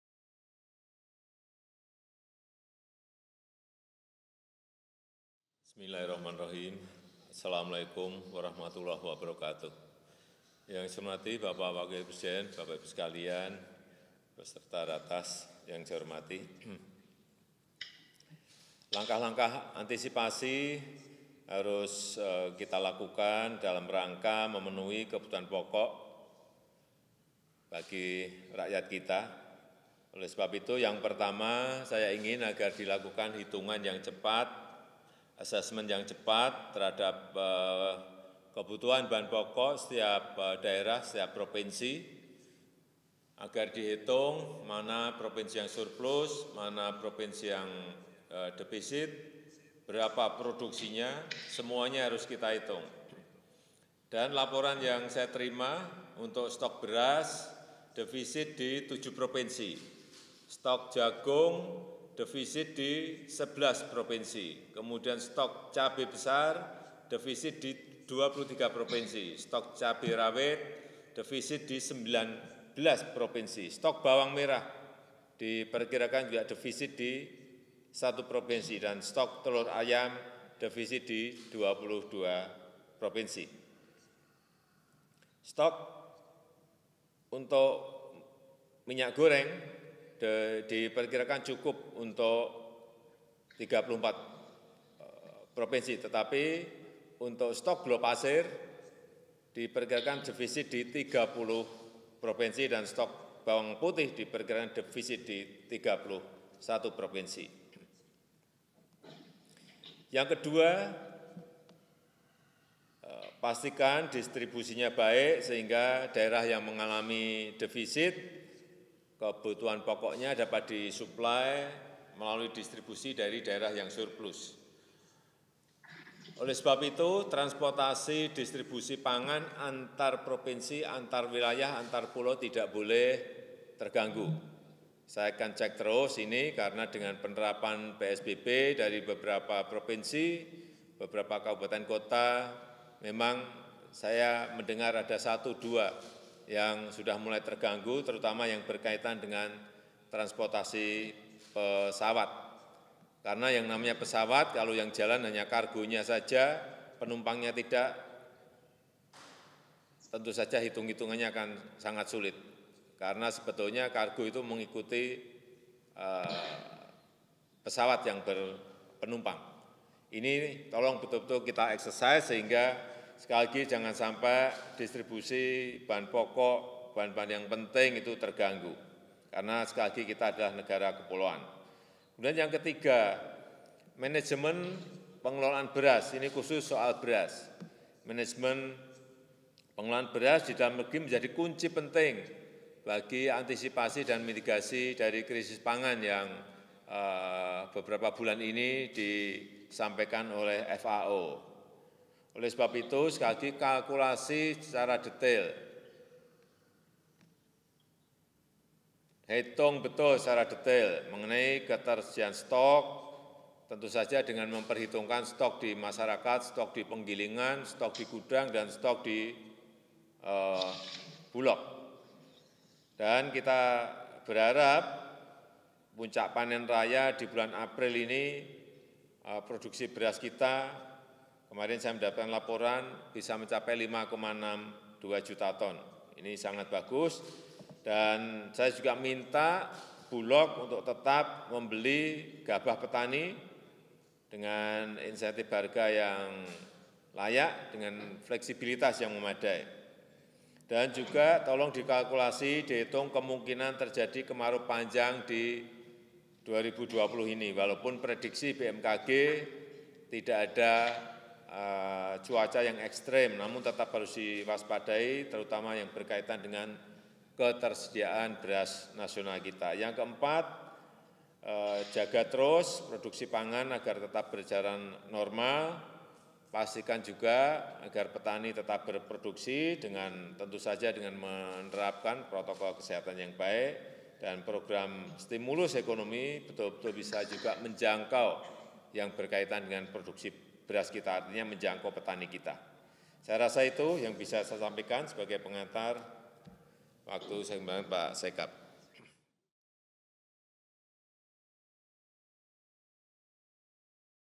Rapat Terbatas Lanjutan Pembahasan Antisipasi Kebutuhan Bahan Pokok, Istana Bogor, 28 April 2020
*Presiden: Manajemen dan Distribusi Bahan Pokok Tidak Boleh Terganggu*

_Kalkulasi yang cermat dan proses produksi yang tetap menerapkan protokol kesehatan juga penting untuk mengantisipasi kelangkaan pangan_

Presiden Joko Widodo kembali mengingatkan jajarannya untuk melakukan perhitungan cermat dalam rangka memenuhi ketersediaan bahan pokok setiap daerah di tengah pandemi Covid-19 dan ibadah puasa Ramadan. Selain itu, diperlukan manajemen distribusi pasokan dan pengelolaan beras serta antisipasi terhadap kemungkinan terjadinya kemarau panjang pada tahun ini.

Demikian disampaikan oleh Presiden saat memimpin rapat terbatas mengenai tindak lanjut antisipasi kebutuhan bahan pokok yang digelar melalui telekonferensi dari Istana Kepresidenan Bogor, Jawa Barat, pada Selasa, 28 April 2020.

"Saya ingin agar dilakukan hitungan yang cepat terhadap kebutuhan bahan pokok setiap daerah, setiap provinsi, agar dihitung mana provinsi yang surplus, mana provinsi yang defisit, dan berapa produksinya. Semuanya harus kita hitung," ujarnya.

Laporan terbaru yang diterima oleh Presiden menyebut bahwa sejumlah komoditas diprediksi masih mengalami defisit di sejumlah provinsi. Di antaranya beras yang diperkirakan defisit di 7 provinsi, jagung di 11 provinsi, bawang merah di 1 provinsi, dan telur ayam di 22 provinsi.

"Stok untuk minyak goreng diperkirakan cukup untuk 34 provinsi, tetapi untuk stok gula pasir diperkirakan defisit di 30 provinsi dan stok bawang putih diperkirakan defisit di 31 provinsi," imbuhnya.

Berkaitan dengan hal tersebut, distribusi pasokan bahan pokok antarwilayah merupakan salah satu hal krusial yang harus diperhatikan. Dengan manajemen distribusi yang baik dan tidak terganggu, daerah-daerah yang memiliki surplus terhadap komoditas tertentu dapat mendistribusikan komoditas tersebut ke daerah-daerah sekitar yang membutuhkannya.

"Saya akan cek terus ini karena dengan penerapan PSBB dari beberapa provinsi, beberapa kabupaten/kota, memang saya mendengar ada satu atau dua yang sudah mulai terganggu," kata Presiden.

Sementara itu, Kepala Negara berpendapat bahwa manajemen pengelolaan beras yang baik menjadi kunci penting dalam mengantisipasi potensi kelangkaan pangan yang dilaporkan oleh Organisasi Pangan dan Pertanian (FAO) PBB. Maka itu Presiden sekali lagi meminta jajarannya untuk turut memperhitungkan ketersediaan stok beras termasuk stok yang ada di masyarakat, penggilingan, gudang, dan Bulog.

"Kita berharap puncak panen raya di bulan April ini produksi beras kita, kemarin saya mendapat laporan, bisa mencapai 5,62 juta ton. Ini sangat bagus dan saya juga minta Bulog untuk tetap membeli gabah petani dengan insentif harga yang layak, dengan fleksibilitas yang memadai," tuturnya.

"Juga tolong dikalkulasi kemungkinan terjadi kemarau panjang di 2020 ini. Walaupun prediksi BMKG tidak ada cuaca yang ekstrem, namun tetap harus diwaspadai terutama yang berkaitan dengan ketersediaan beras nasional kita," imbuh Presiden.

Terakhir, Presiden Joko Widodo menginstruksikan agar aktivitas produksi komoditas pangan dapat tetap berjalan normal di tengah kebijakan tanggap darurat dan pembatasan sosial berskala besar di sejumlah daerah.

"Tentu saja dengan menerapkan protokol kesehatan yang baik. Program stimulus ekonomi juga harus bisa menjangkau yang berkaitan dengan produksi beras kita yang artinya turut menjangkau petani kita," tandasnya.